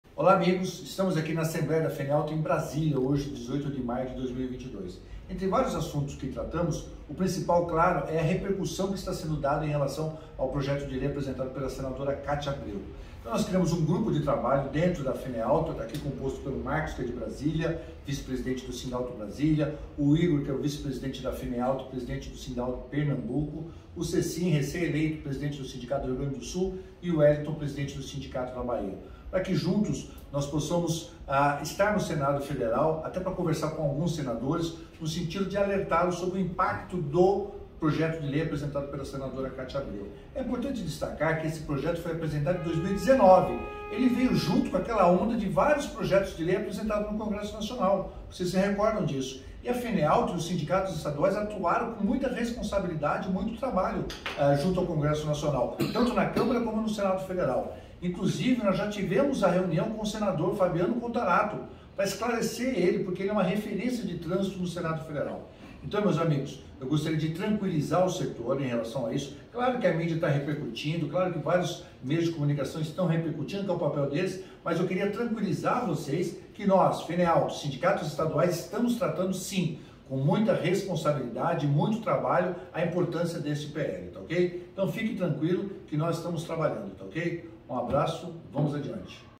PRESIDENTE DA FENEAUTO COMENTA PROJETO DE LEI DA SENADORA KATIA ABREU
O presidente da Feneauto, Magnelson Carlos de Souza, acompanhado de representantes do setor, gravou vídeo falando sobre o PL da Senadora Katia Abreu.